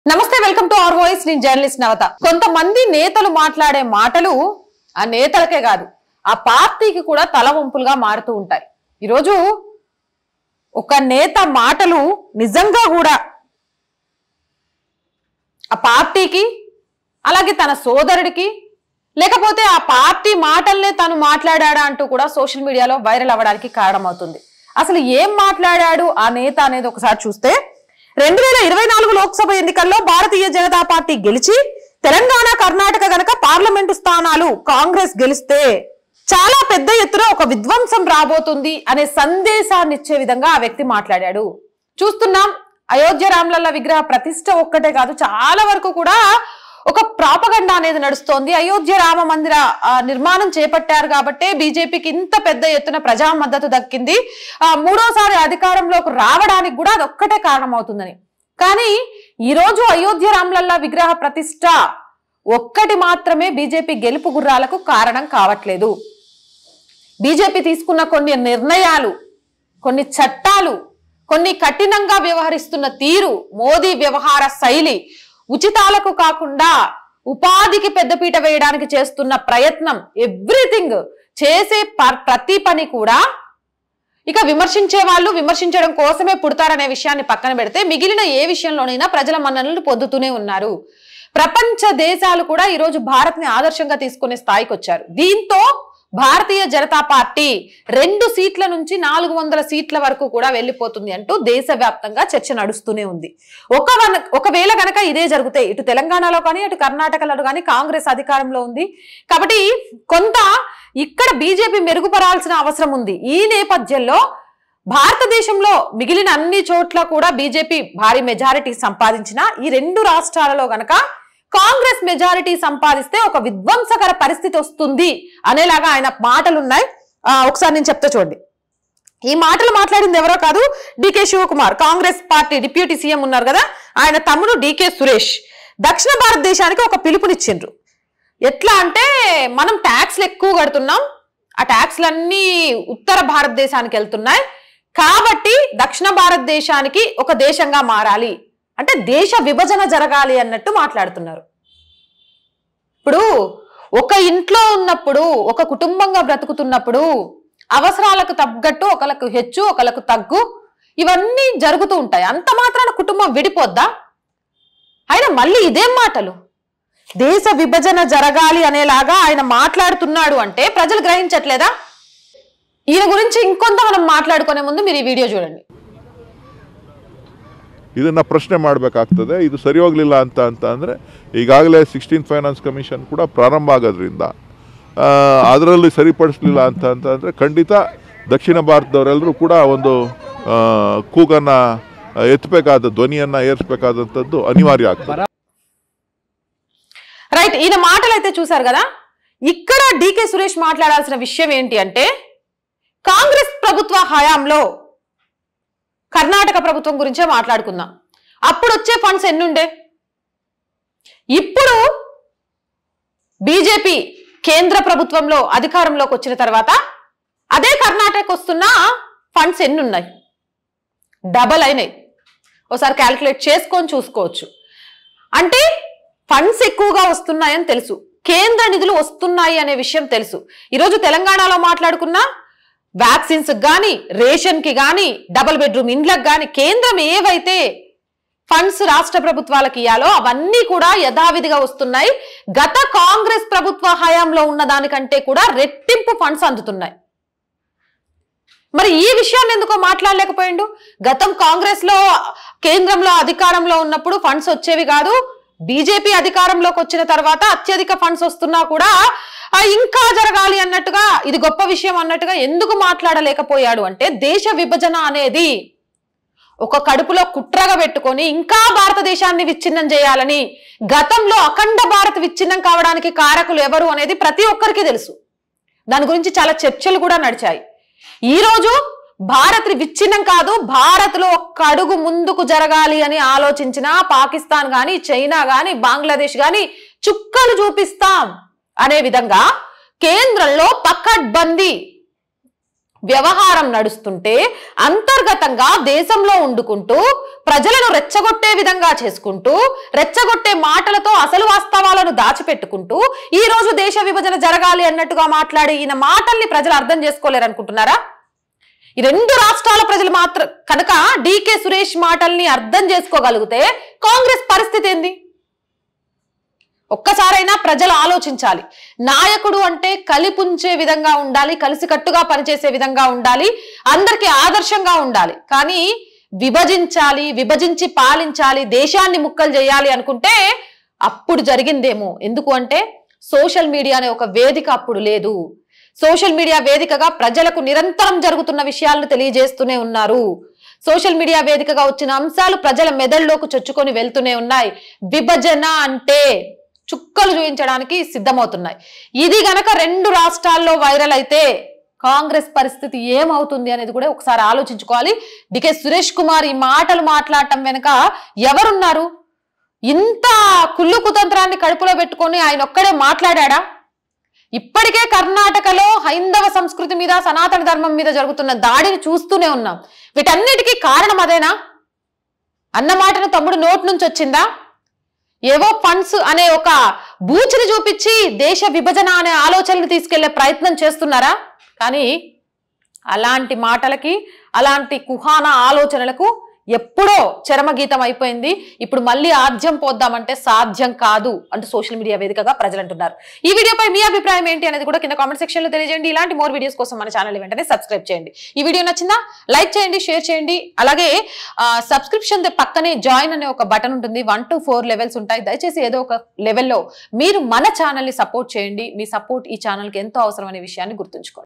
కాంగ్రెస్ గెలవకుంటే దేశ విభజన....మళ్ళీ కుట్ర మొదలా..? | Karnataka Congress Government | R Voice
Please Support us :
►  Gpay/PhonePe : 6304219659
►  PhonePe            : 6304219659